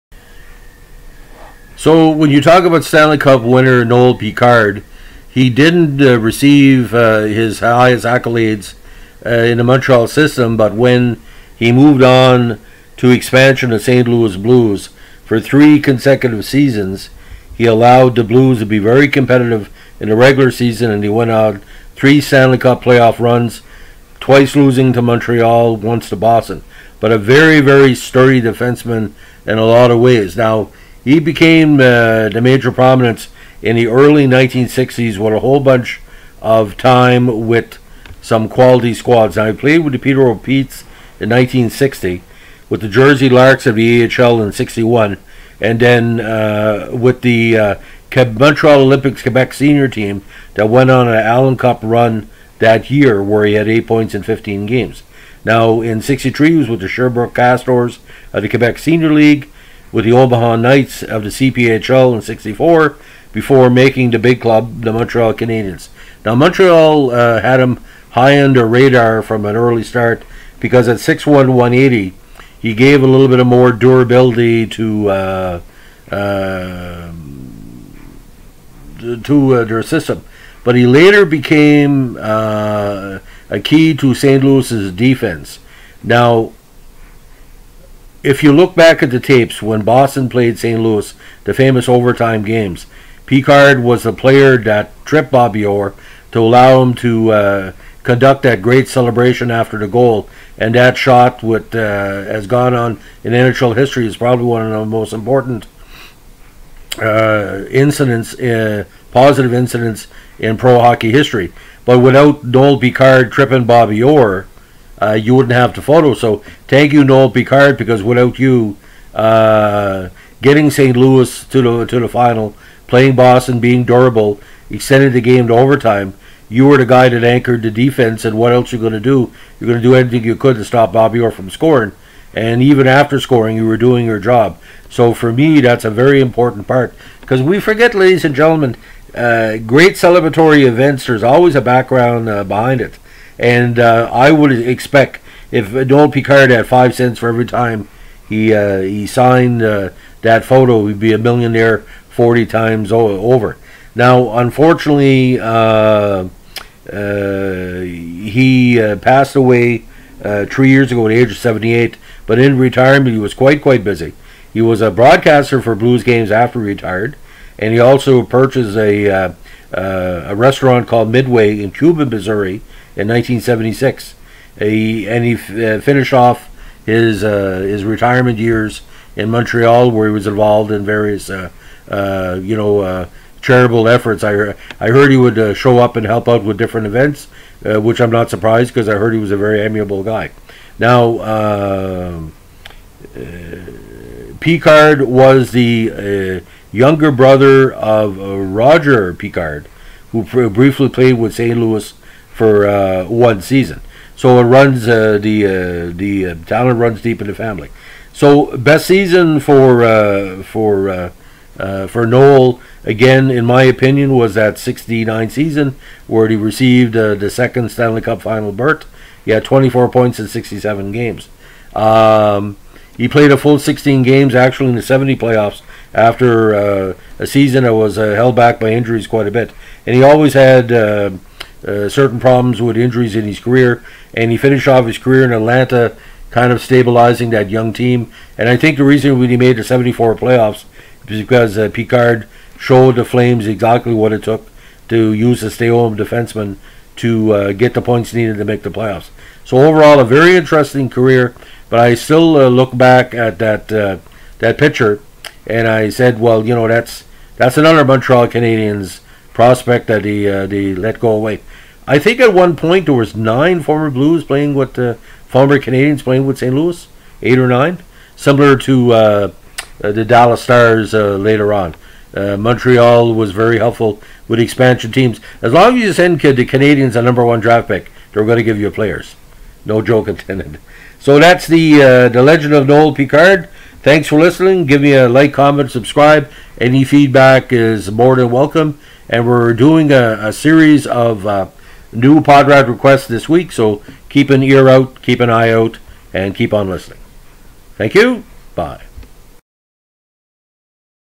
The Many Lives of Noel Picard
Cup winner with Habs later went on to three straight NHL final appearances with the Blues